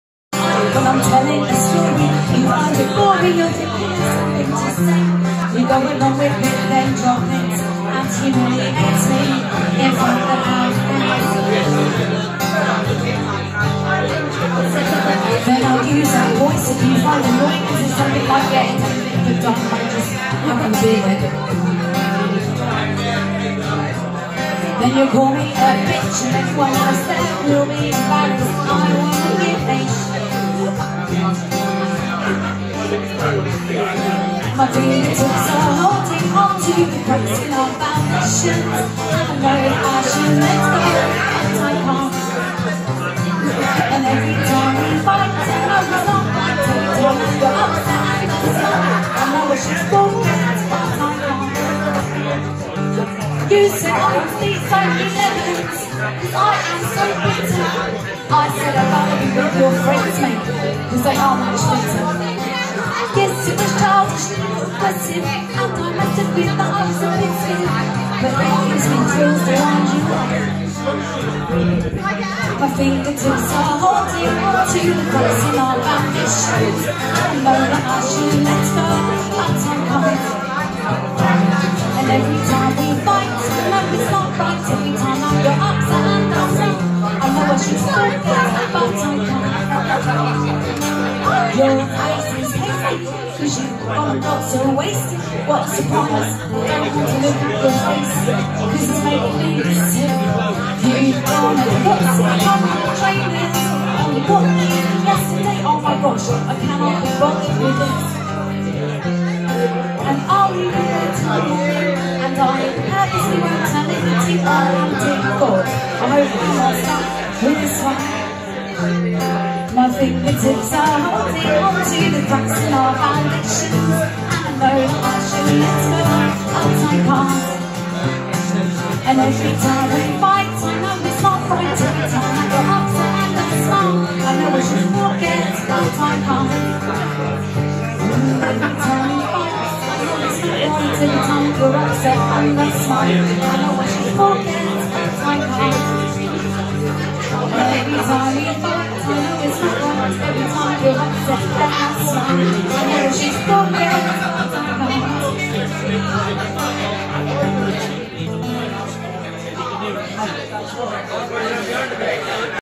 Dune Acoustic 2021
Dune Acoustic live August 28th 2021 at the dolphin pub Canterbury